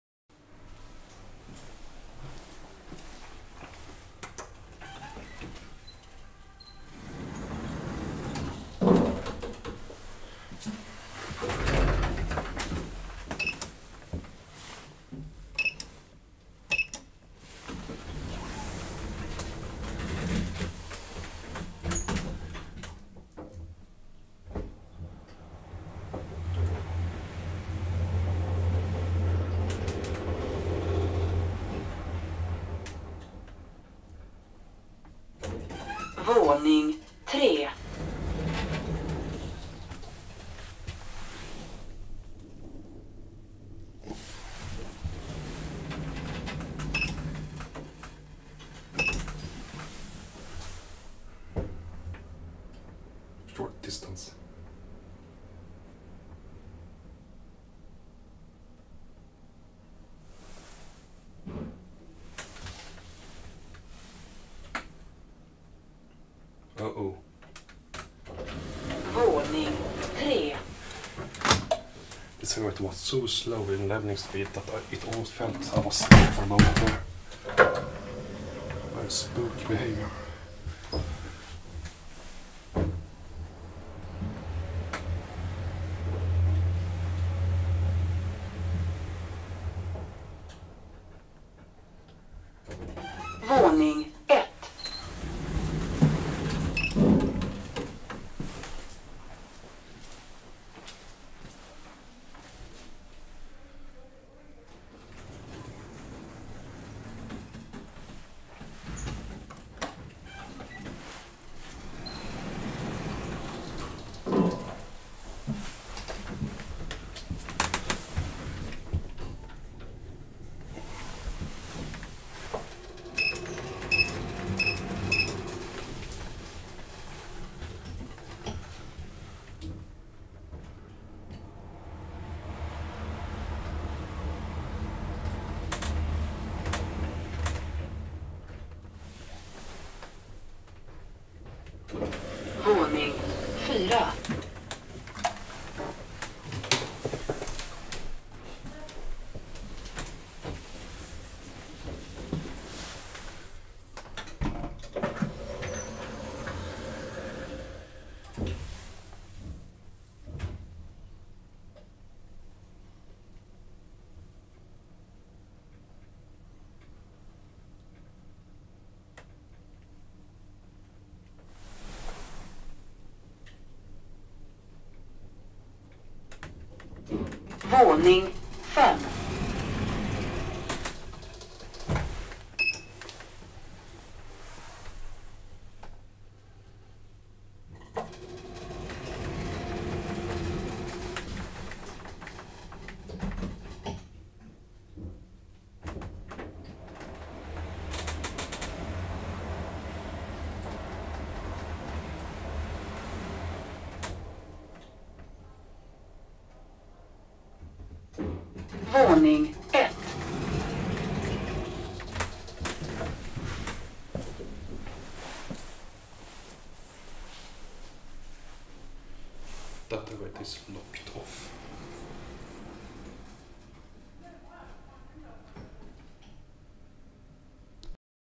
2x Svenska Hiss (Magnusson mod.) Traction Elevators @ Gustavslundsvägen 141B, Stockholm, Sweden!
ELEVATOR INFORMATION!

Manufacturer/Tillverkare: Svenska Hiss AB
Model/Modell:
Installation year/Byggår: ~1987
Current modernizer/Senaste modernisering av: Magnussons Hiss AB
Latest modernization year/Senast moderniserat år:
Current model/Nuvarande modell: Hissmekano
Serial Number/Tillverkningsnummer: 87 N 16170 & 87 N 16171
Capacity/Maxvikt: 630Kg - 8 Persons
Floors/Antal våningar: 7 = E1 D1 E2 D2 E3 (D3) (E4)
Cab refurbished in ~2013 by KONE

Location/Adress: Alviks strand | Hiss E4 & E5 | Gustavslundsvägen 141, 167 51 Bromma (Stockholm/Alvik) SWEDEN!